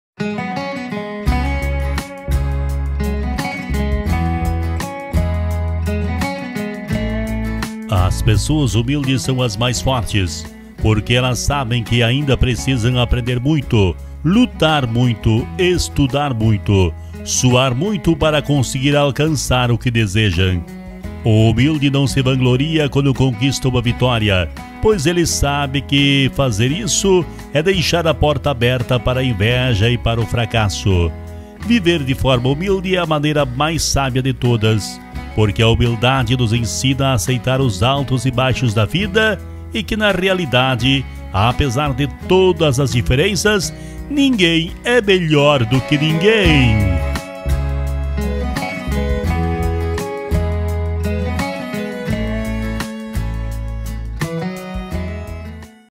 Uma Vida Humilde, por Valdir França
Uma Vida Humilde, mensagem de caráter positivo e alentador, na voz do jornalista e comunicador Valdir França.

Gestão do Canal: Mídiaplay
Jornalista/Editor Responsável: Valdir França - MTB/RS 19.605